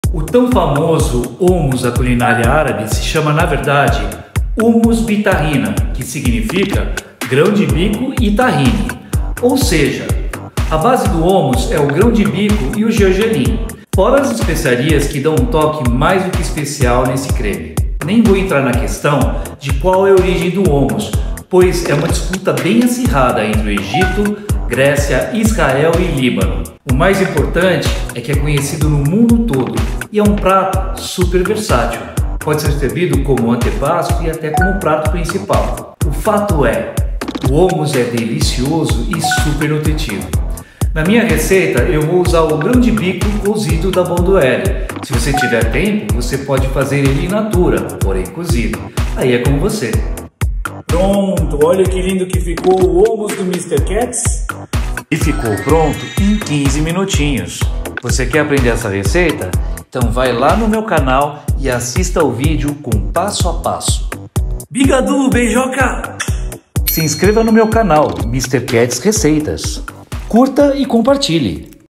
FIQUE LIGADO NA ESTRÉIA DO HOMUS SUPER RÁPIDO DO MISTER KATS
O tão famoso homus da culinária árabe se chama na verdade humos bi tahina que significa grão de bico com tahine. Ou seja, a base do Homus é o grão de bico e gergelim, fora as especiarias que dão um toque mais do que especial a esse creme. Nem vou entrar na questão de qual é a origem do Homus, pois é uma disputa entre Egito, Grécia, Israel e Líbano. O mais importante que é conhecido no mundo todo e é um prato super versátil. Pode ser servido com um antepasto e até como prato principal. O fato é que é delicioso e super nutritivo. Na minha receita eu uso o grão de bico cozido da Bonduelle, mas claro, se você tiver tempo, pode fazer com ele in natura, porém cozido. Aí é com você! Bigaduuuuu! Beijoca!

Curta e compartilhe!
Bigaduuuuu!!!